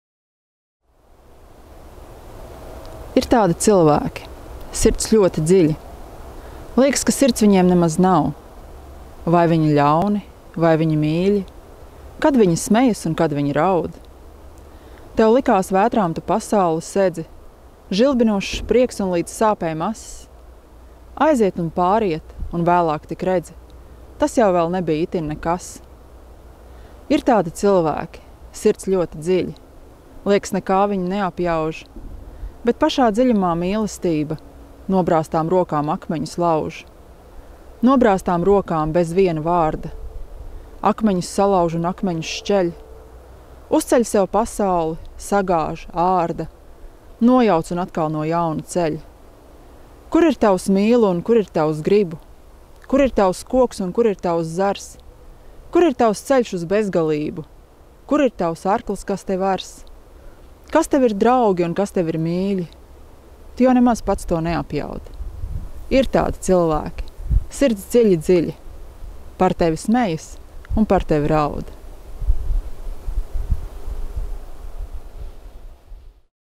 42 no 88
Imanta Ziedoņa dzejoļi no galvas līdz gada beigām.